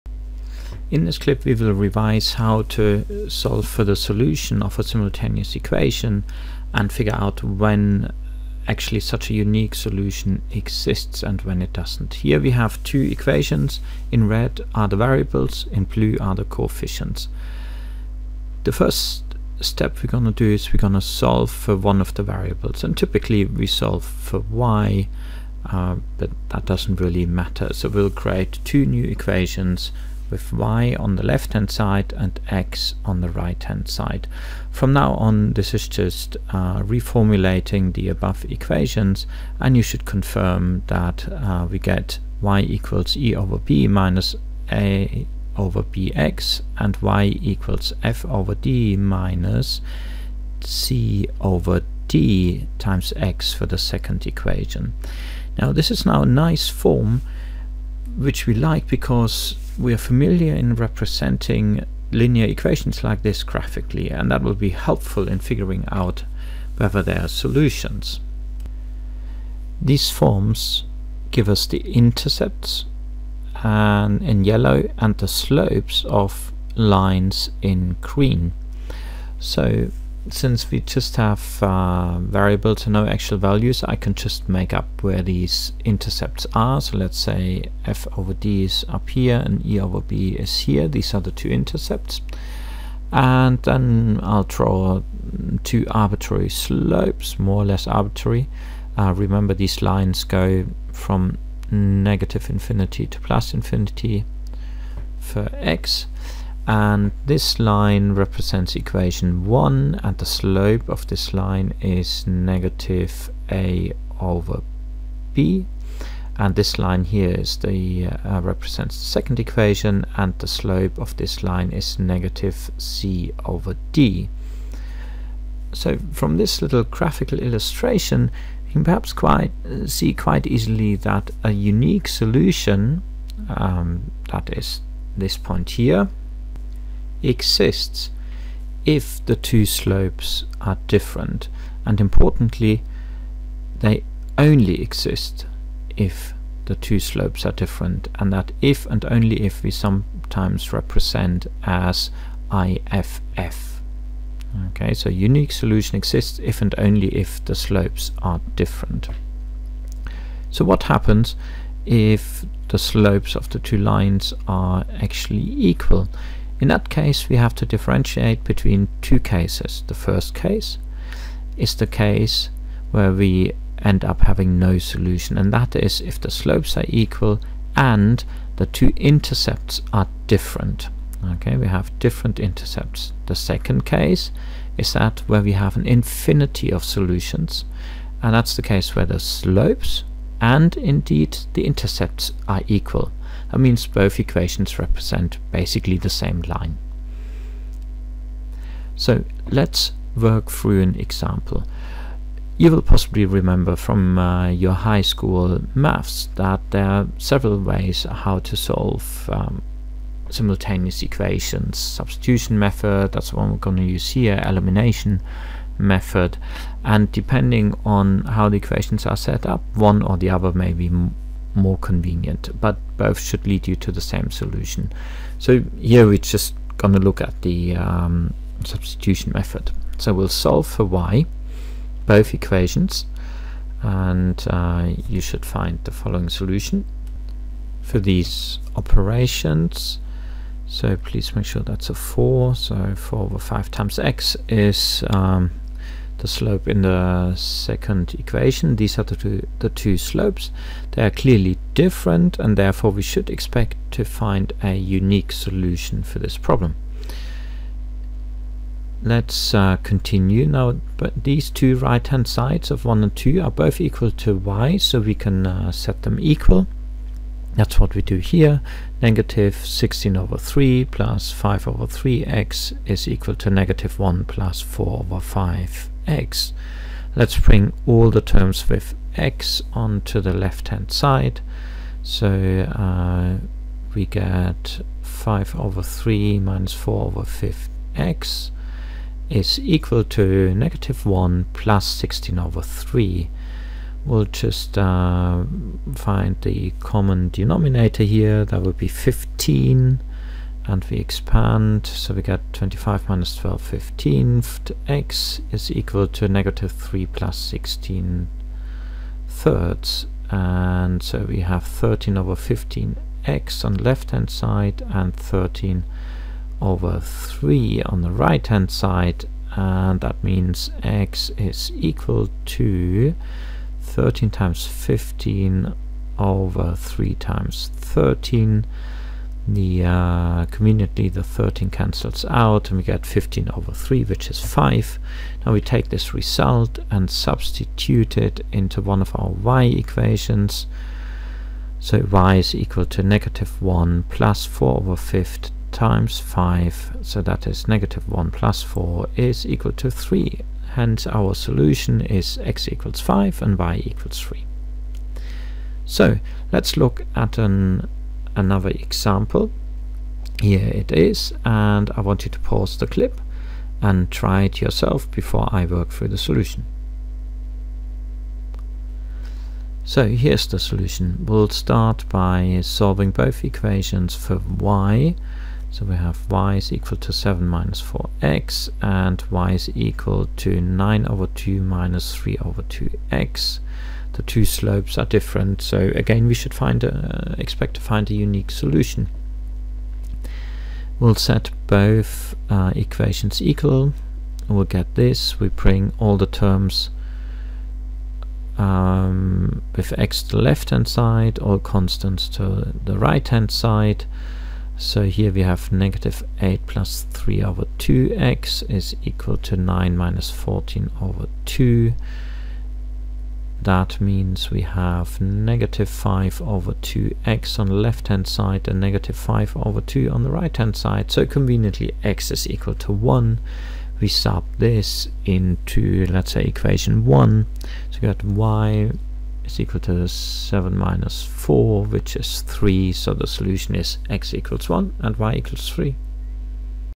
SimEquation
Simultaneous equations, Existence of solutions and substitution method.